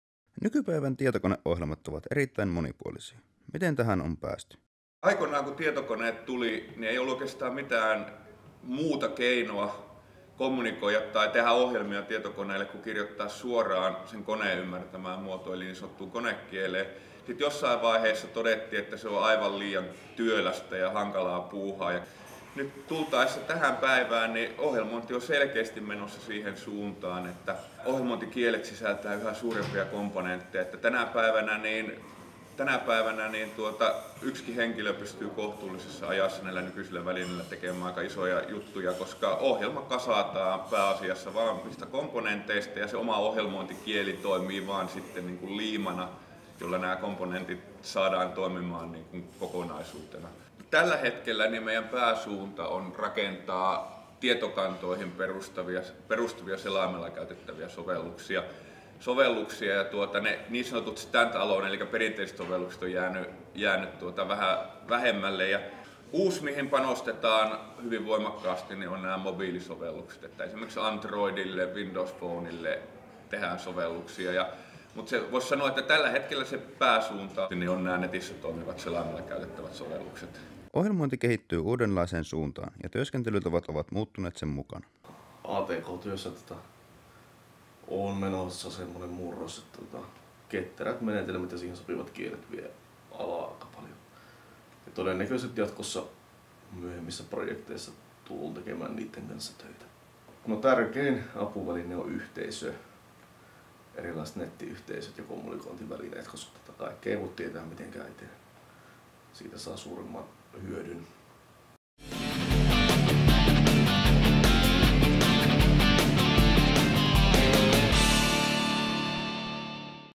MAMK tietojenkäsittelyn koulutusohjelma ohjelmointitapojen tulevaisuus
Ohjelmointi ja ohjelmointitavat ovat nykyisin kokoelma monipuolisia ohjelmistoja ja työtapoja. MAMK:n tietojenkäsittelyn koulutusohjelmassa tulevat tutuksi molemmat.